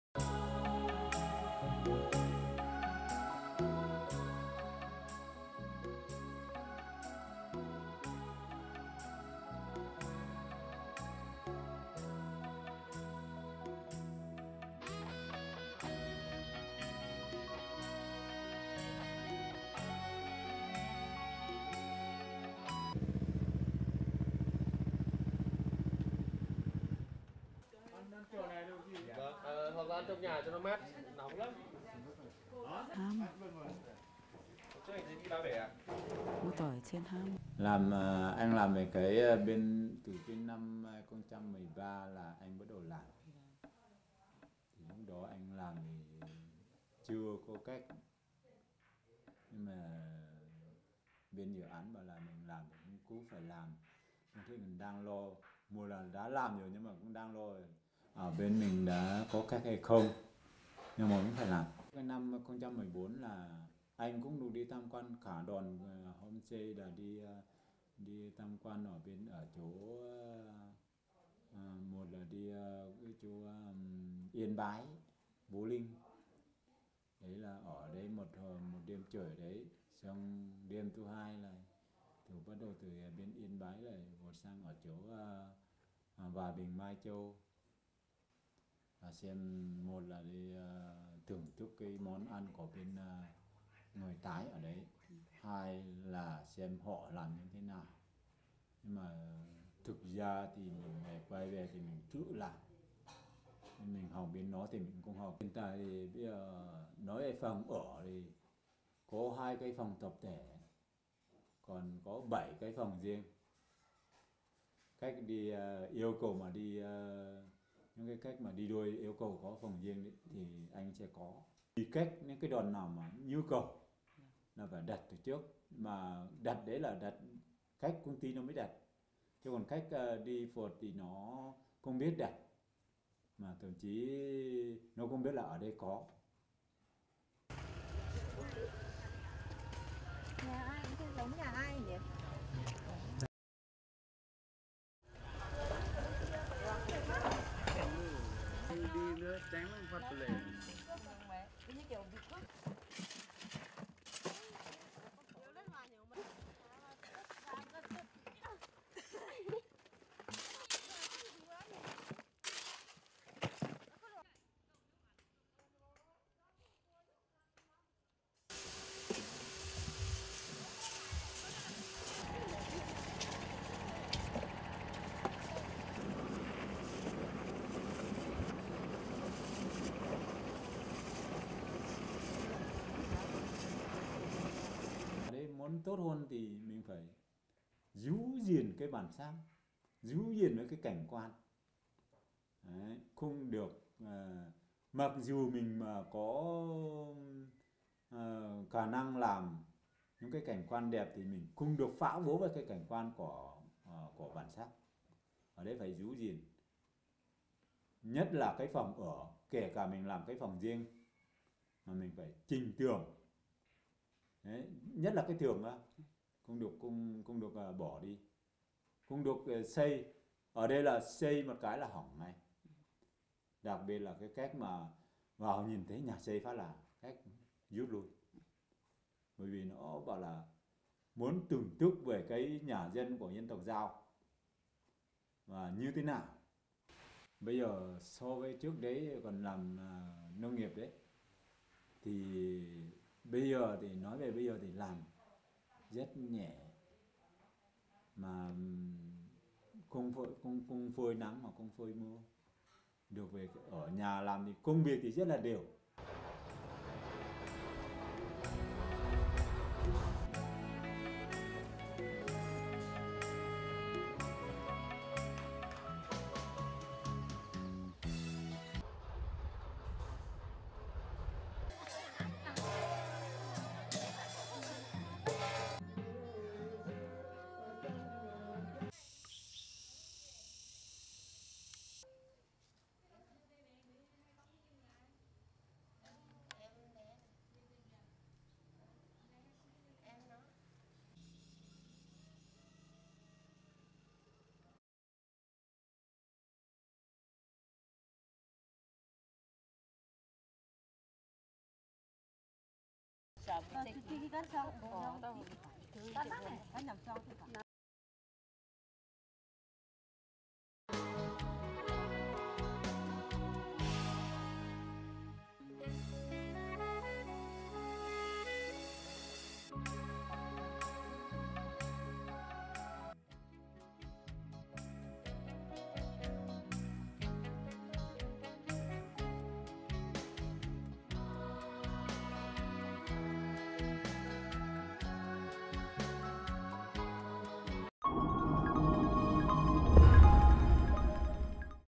Hà Giang: Thung lũng "thiên đường" của người Dao
Thôn Nặm Đăm, xã Quản Bạ, huyện Quản Bạ (Hà Giang) nằm yên bình trong một thung lũng thơ mộng dưới chân núi đôi nổi tiếng. Toàn thôn có 60 hộ, 288 khẩu đều là người dân tộc Dao. Trong những năm qua, nhờ phát triển du lịch cộng đồng kết hợp trồng cây thuốc quý, đời sống của người Dao nơi đây ngày một khấm khá.